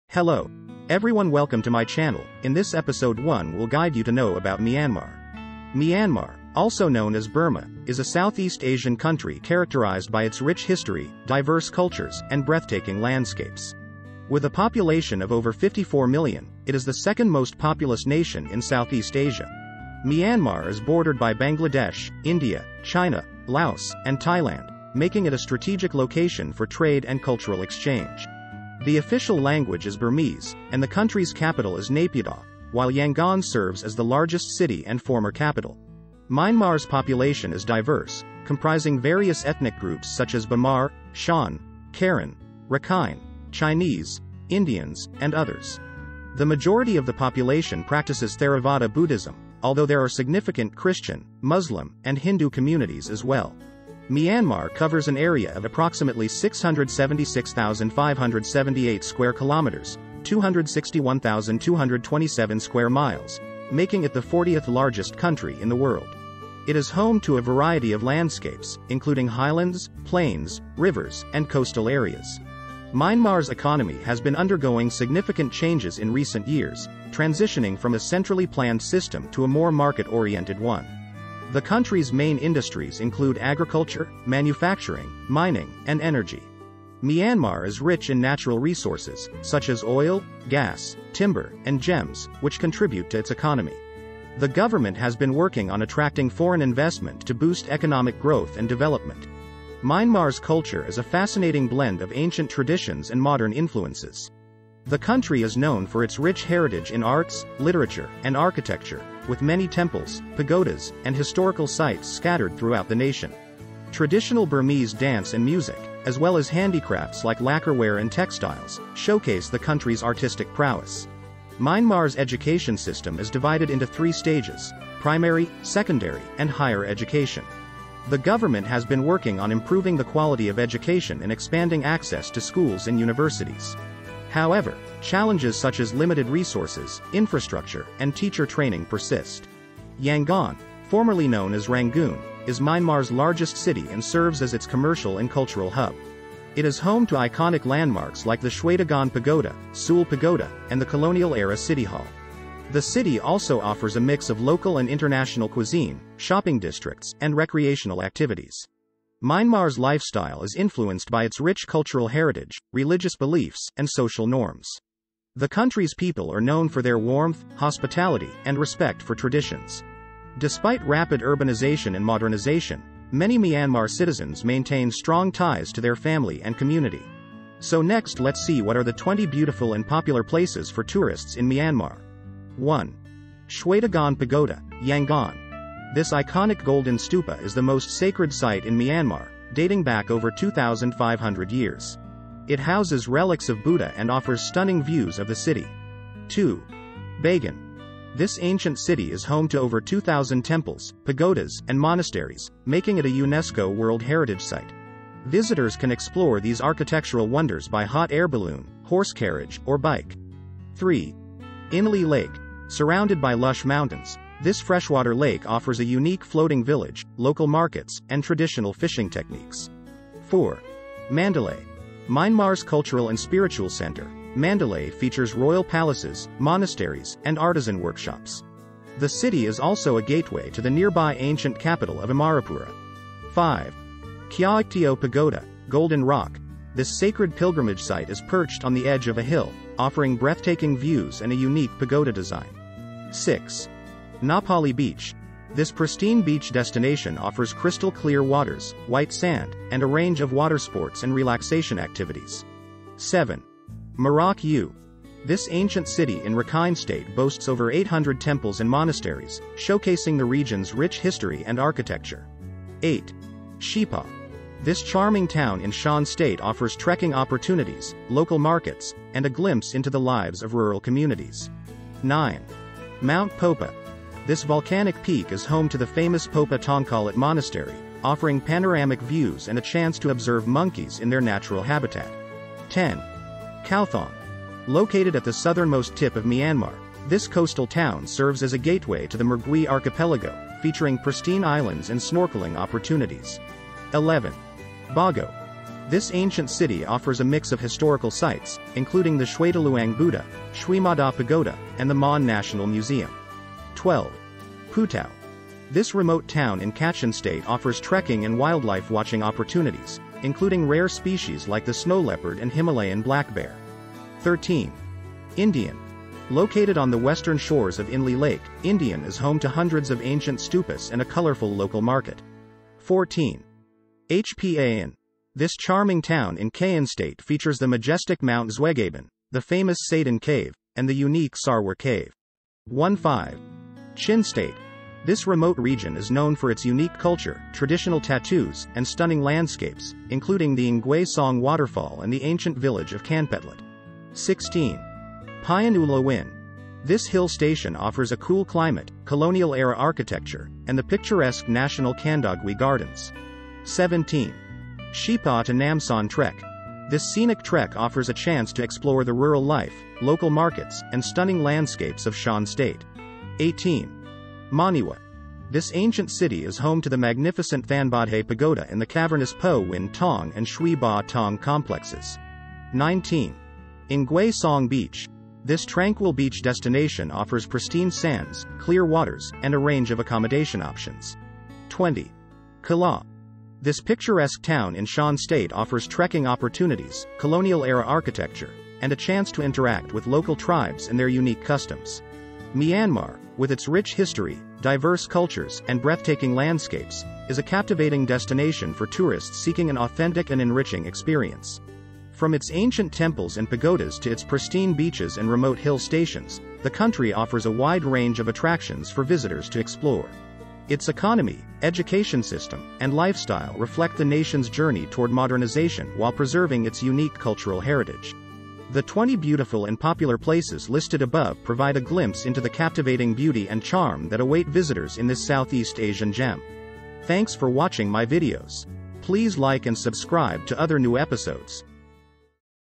Episode 16 - Myanmar & 20 Most Beautiful Place in Myanmar ប្រទេសមីយ៉ាន់ម៉ា(ប្រទេសភូមា)សម្រាប់ទេសចរណ៍
Myanmar, a country in Southeast Asia, is a land of ancient temples, lush green hills, and warm hospitality. Located between India and Thailand, Myanmar has a rich cultural heritage and a turbulent history. The country was under military rule for decades, but in 2011, democratic reforms began to take shape. Today, visitors can explore the magnificent Shwedagon Pagoda in Yangon, the former capital, or visit the ancient city of Bagan, where thousands of temples dot the landscape. The Inle Lake is a must-visit destination, with its floating gardens and traditional wooden boats. The people of Myanmar are known for their friendly nature and love for festivals. The country is also home to diverse wildlife, including elephants, tigers, and orangutans. Despite its challenges, Myanmar is a country with a lot to offer, from its stunning landscapes to its vibrant culture and warm people.

Recorded by Hean Matt-1M's Studio